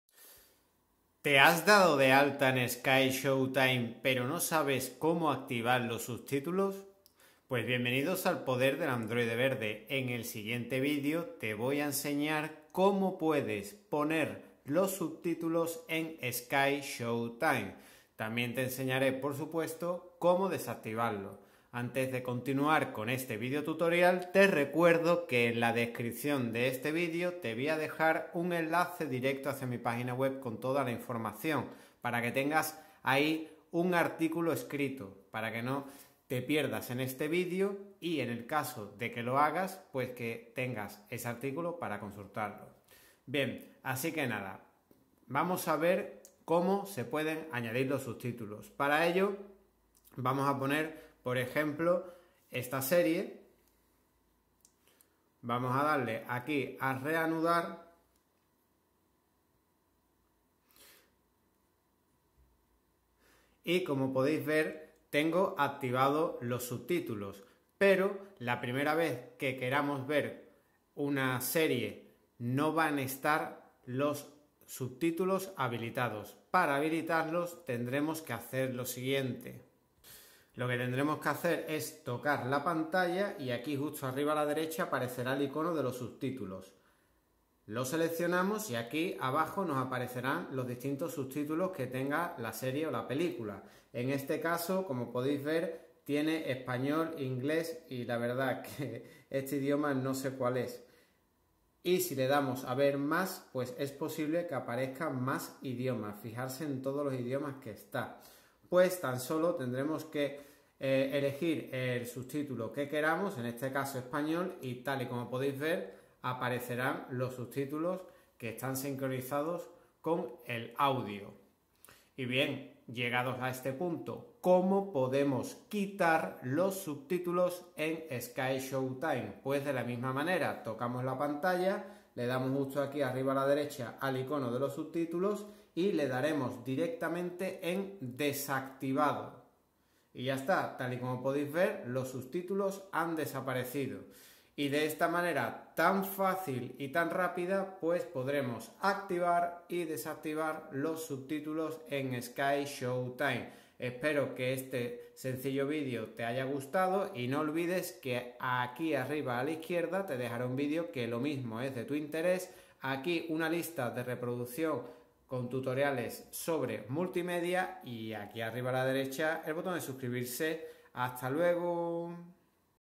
📱🆎 Como PONER o QUITAR SUBTÍTULOS en SKYSHOWTIME FÁCIL y RÁPIDO
¿Te gustaría AGREGAR SUBTÍTULOS en SKYSHOWTIME❓ o ¿quieres DESACTIVARLOS❓. En ESTE ☝ VÍDEO informativo te enseñaré como colocar subtítulos en SkyShowTime paso a paso.
00:00 👉 Introducción
00:46 👉 Como añadir subtítulos en SkyShowTime fácil y rápido
02:03 👉 Como quitar subtítulos en SkyShowTime paso a paso

Vídeos Relacionados 📹

Tutoriales ⚙️

Multimedia 📹